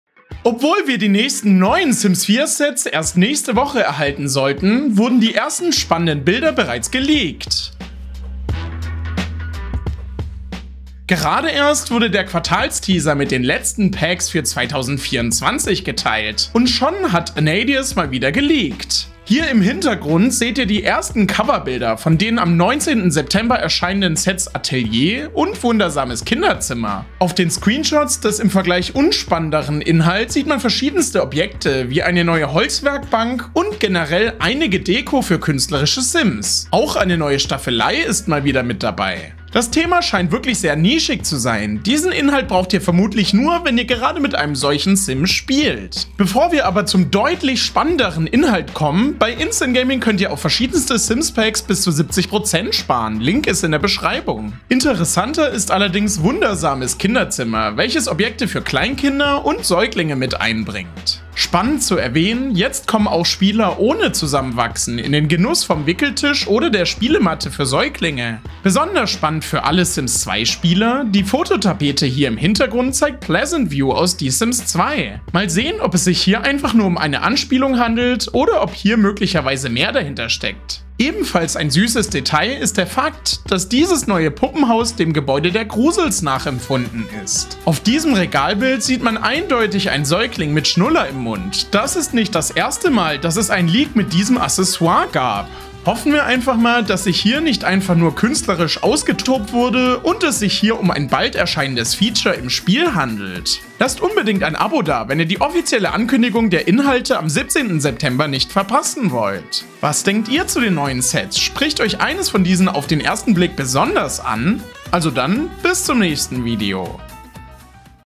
Sims 4 "Atelier"- und "Wundersames Kinderzimmer"-Packs GELEAKED! | Short-News
Die beiden neuen Sets sollten eigentlich erst nächste Woche erscheinen, jetzt gab es aber schon vorbereitende Leaks!